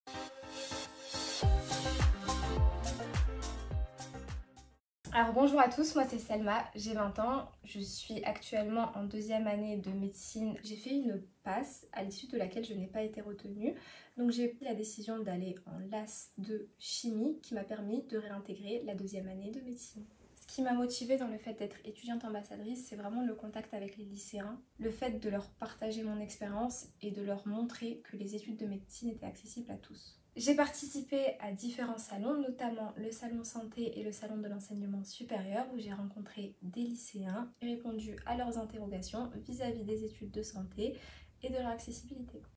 Selma "Ma motivation à être étudiante ambassadrice"
Motivation à être étudiante ambassadrice : présentée par Selma.
Une capsule vidéo réalisée par le SCUIO-IP dans le cadre du projet COMPAS.